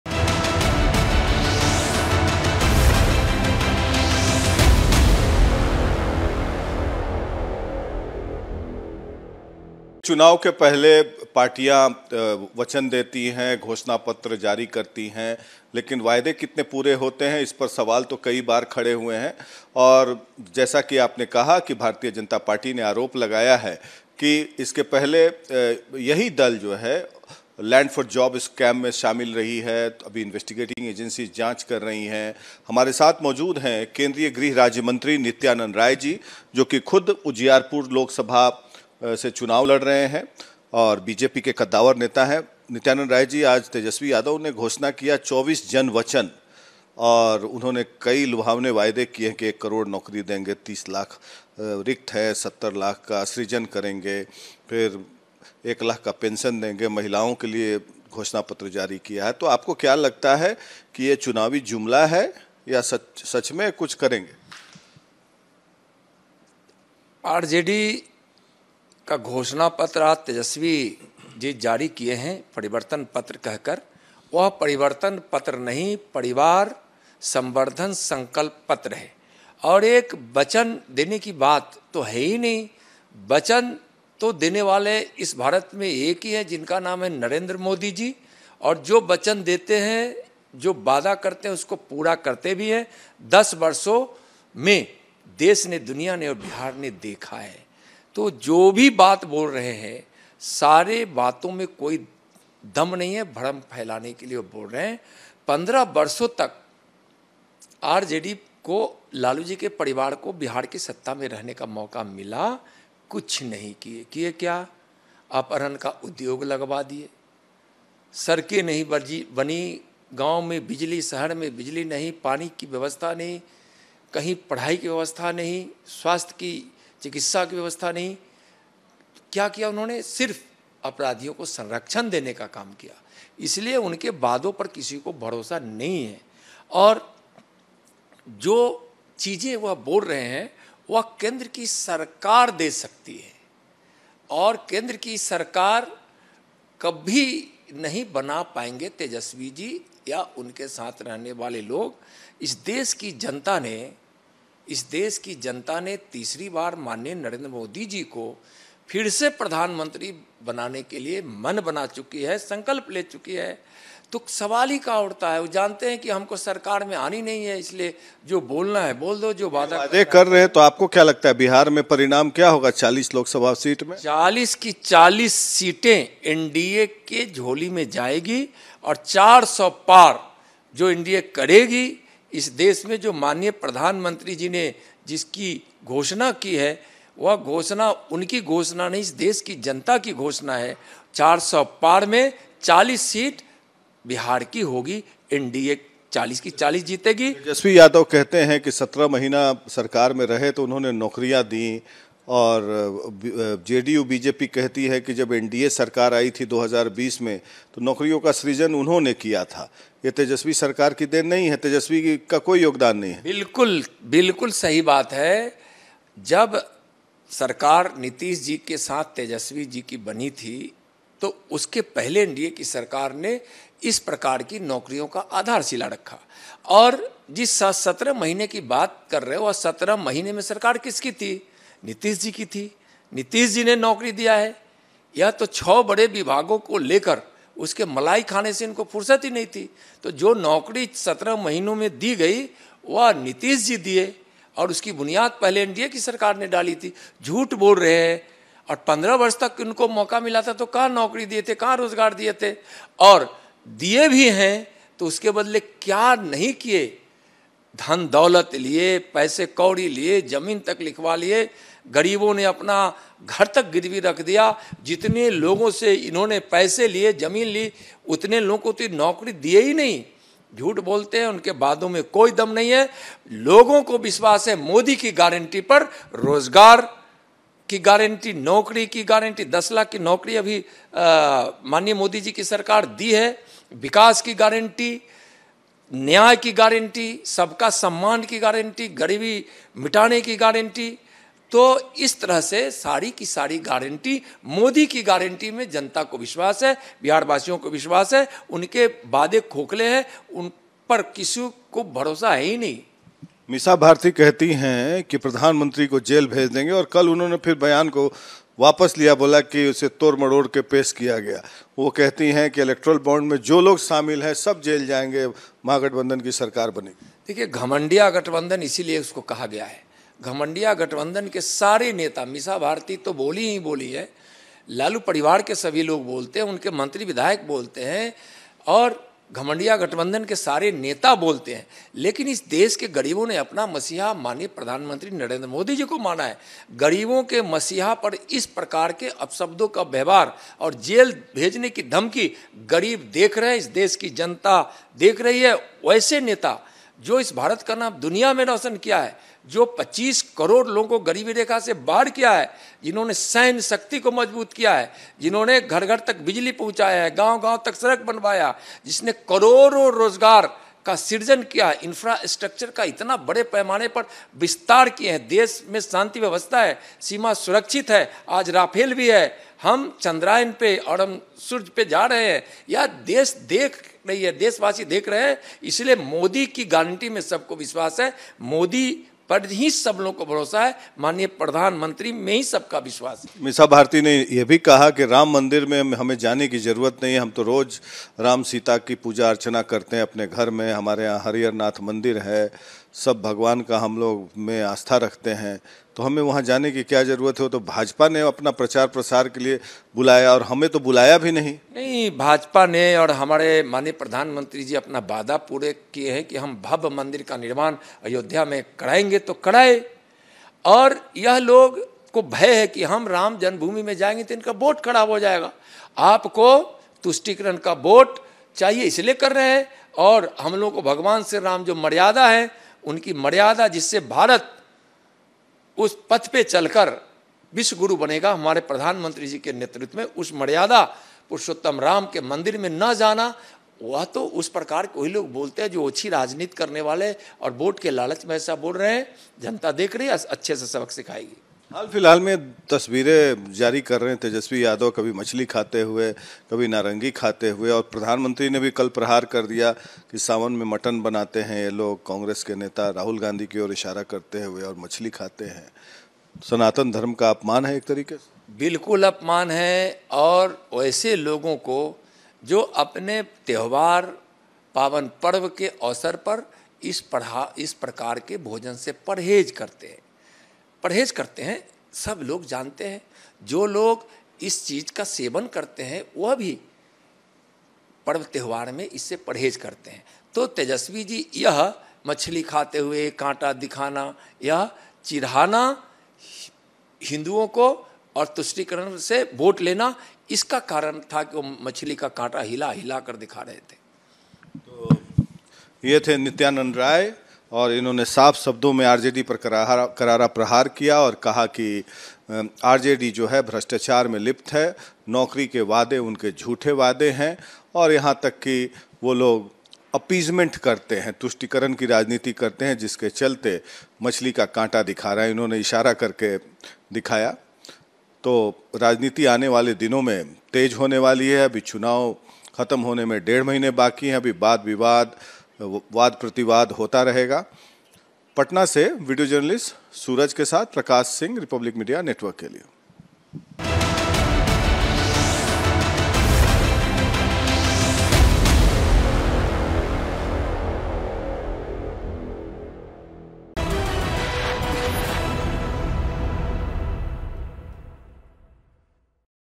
Nityanand Rai ने Lalu परिवार को खूब जलील किया " इन्होंने बिहार को लूटा और अपराधियों का साथ दिया"
Nityanand Rai ने किया तेजस्वी यादव पर प्रहार "वचन देने और पूरा करने वाले भारत में एक ही है वो हैं पीएम मोदी, तेजस्वी की बातों में कोई दम नहीं है, लालू परिवार ने बिहार में कुछ नहीं किया"